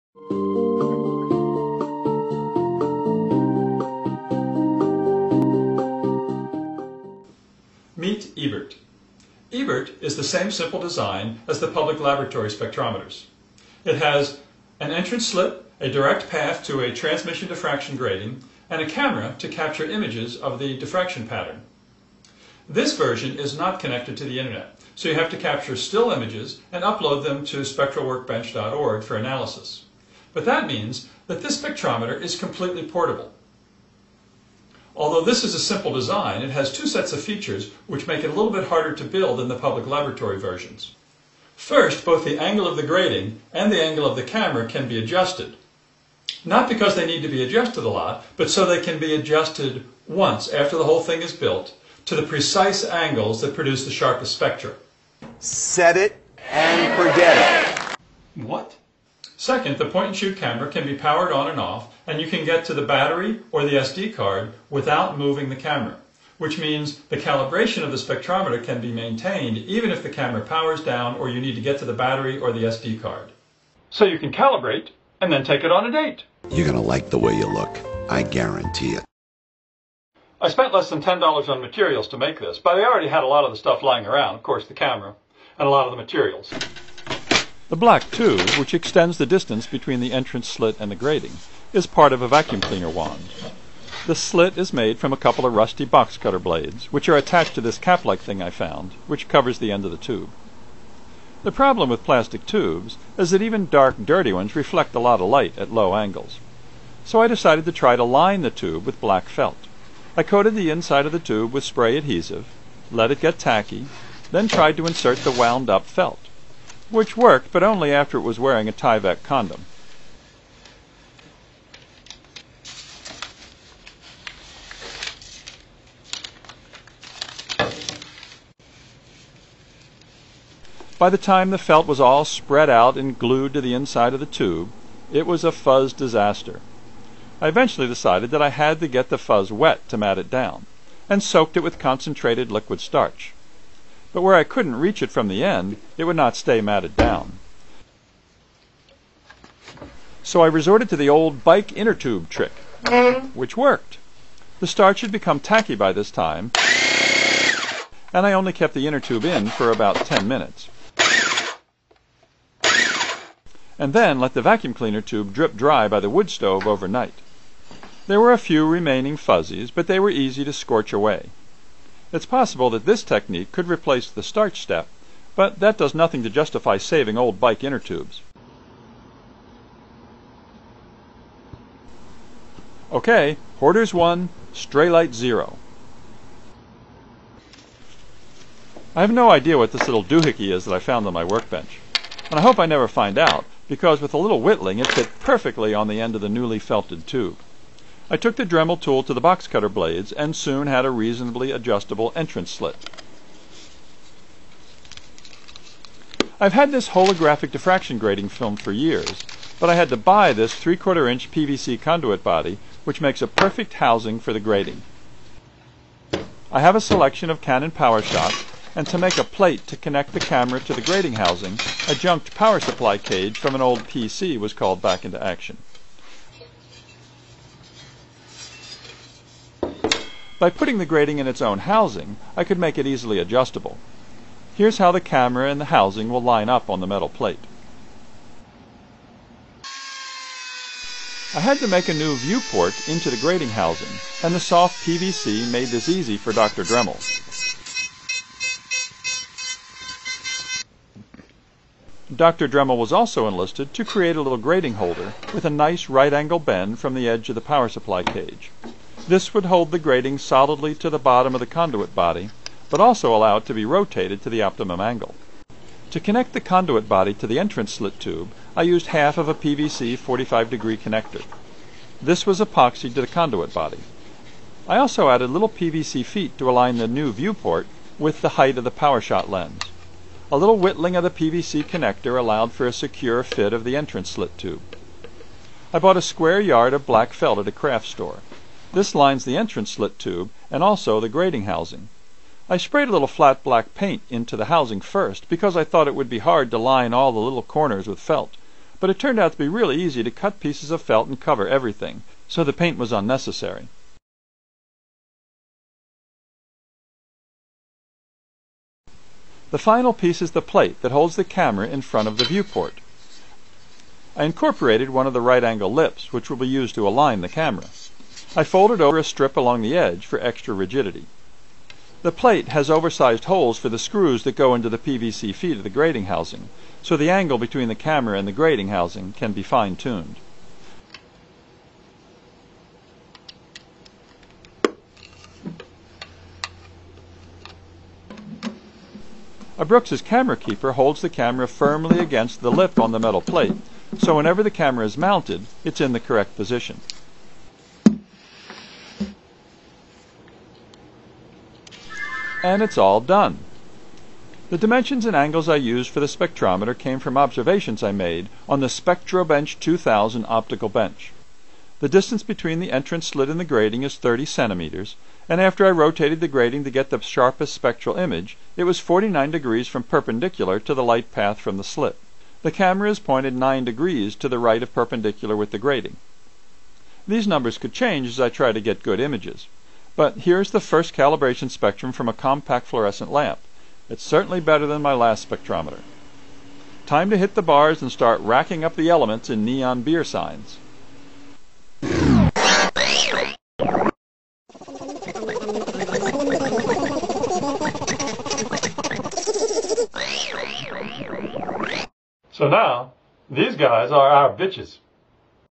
Ebert
Building instructions for a simple spectrometer based on designs described at publiclaboratory.org. More info is